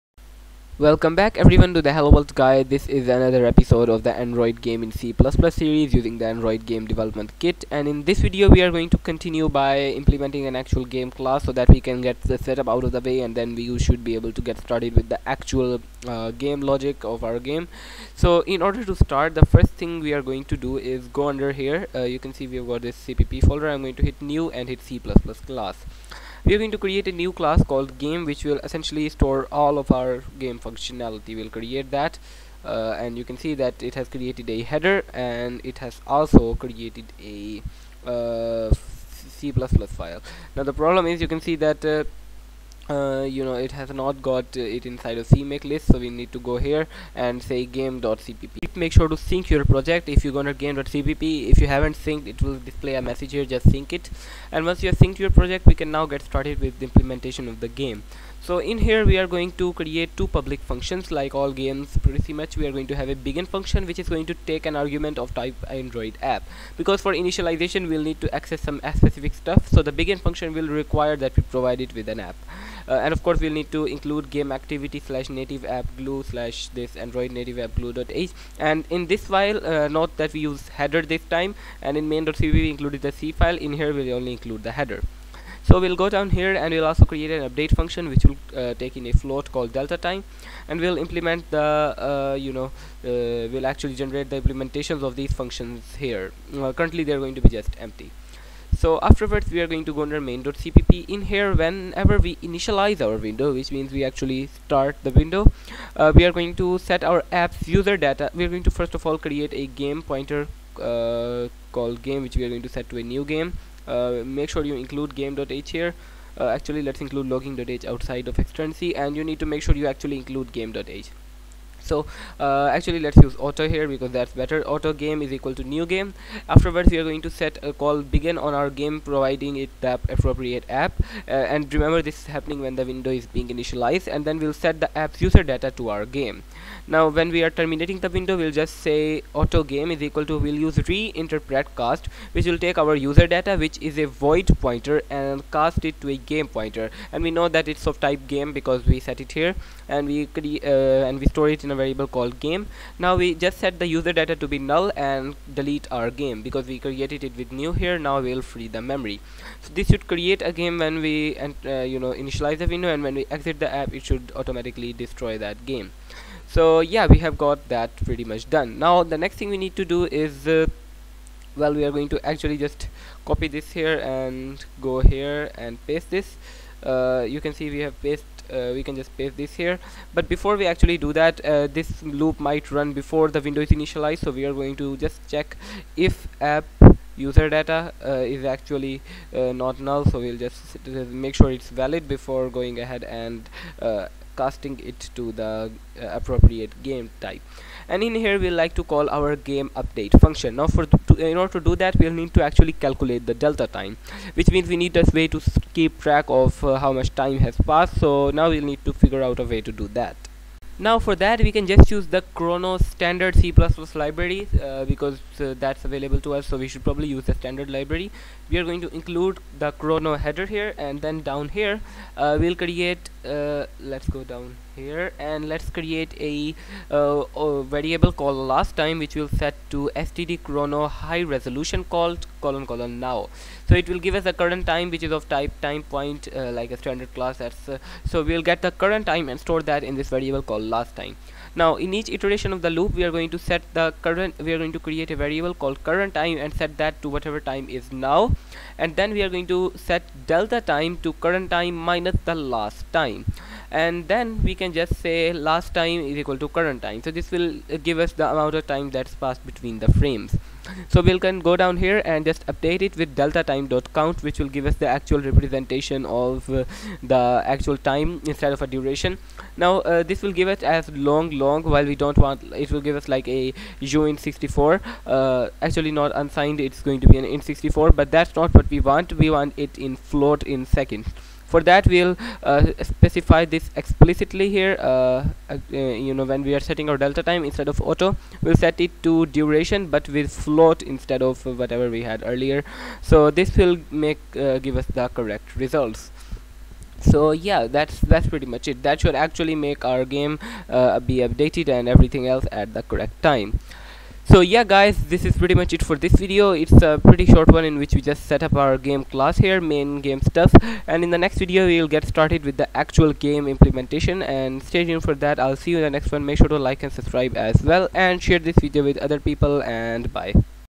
Game Class and Update Loop | Getting Started
Welcome back to the Android Game Development series! In this video, we dive into the core of game development by creating our game class and setting up the main game loop. These fundamental components are crucial for managing game state, handling user input, and rendering game elements.

Topics Covered:

Learn how to structure a game class and understand its role in managing game state.
Explore the concept of a game loop and how it drives the continuous flow of your game.
Dive into the update phase, handling user input, and updating the game state.
Understand the importance of the render phase in displaying game elements on the screen.
Manage frame rates and use delta time to ensure smooth gameplay across different devices.
By the end of this tutorial, you'll have a solid foundation for building your own Android games. Test your newfound knowledge and leave your questions or thoughts in the comments below!

Missed the previous videos? Catch up here: [Link to Previous Video 1] and [Link to Previous Video 2].